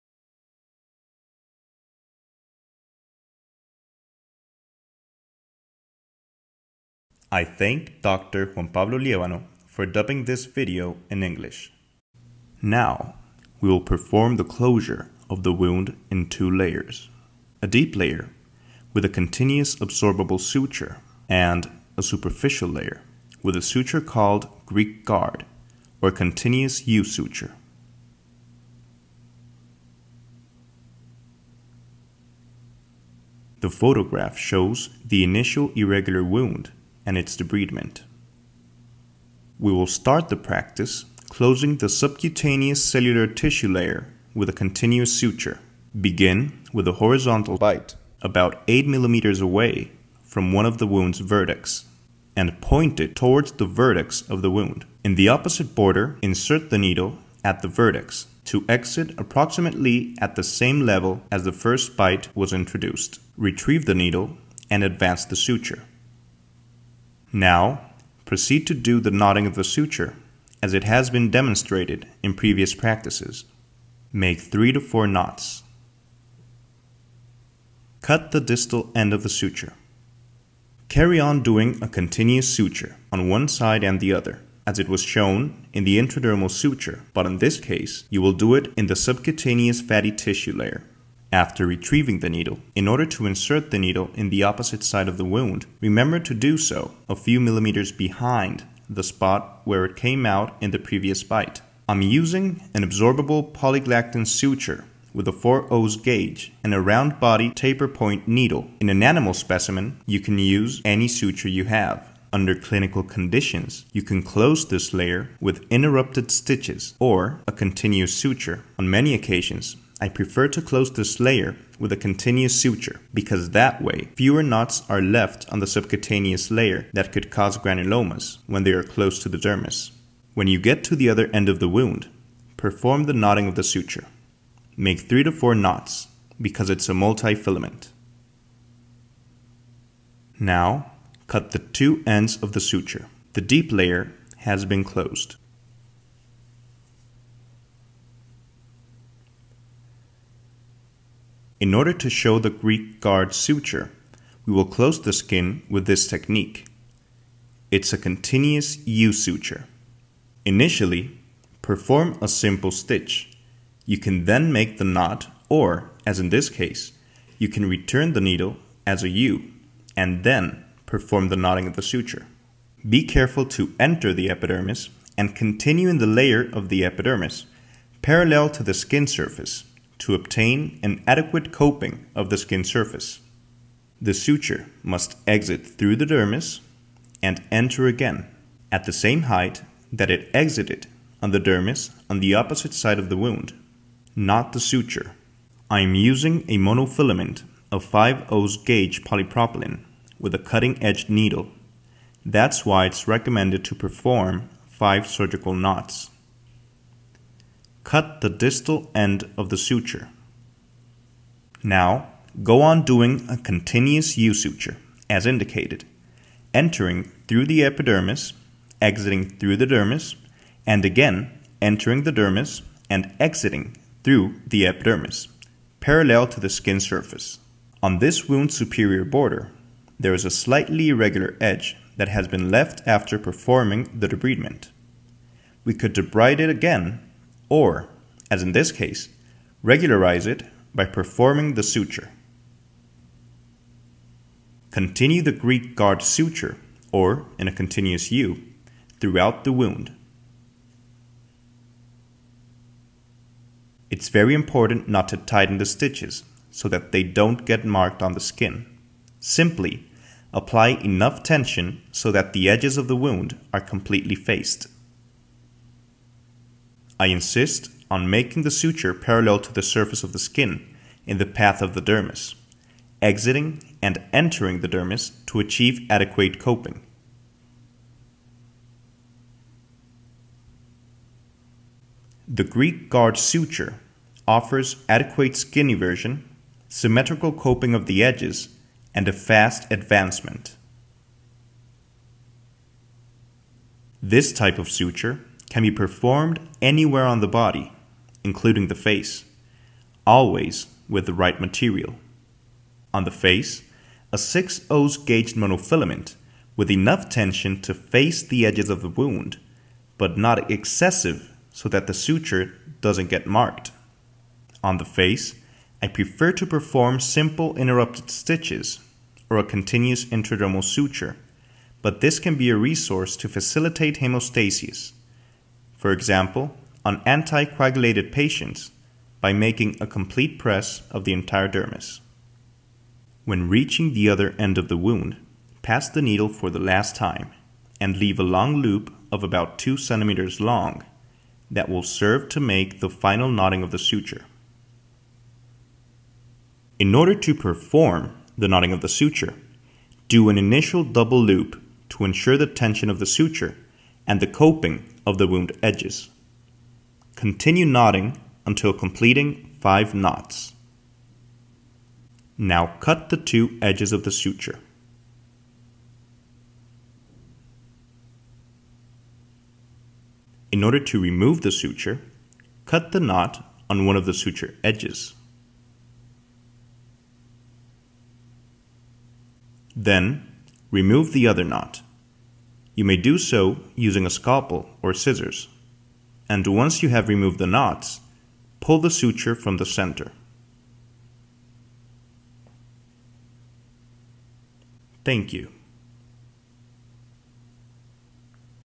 English - Practice # 04: Layered skin closure and “U” Suture, - Dr. Andrés Ferro, Plastic Surgery.
Using the same model we used on the irregular edged wound debridement practice, the technique for performing skin closure in two layers is explained and, when performing skin closure, the Greek Guard suture technique (or "U" suture) is demonstrated, as well as its subsequent removal.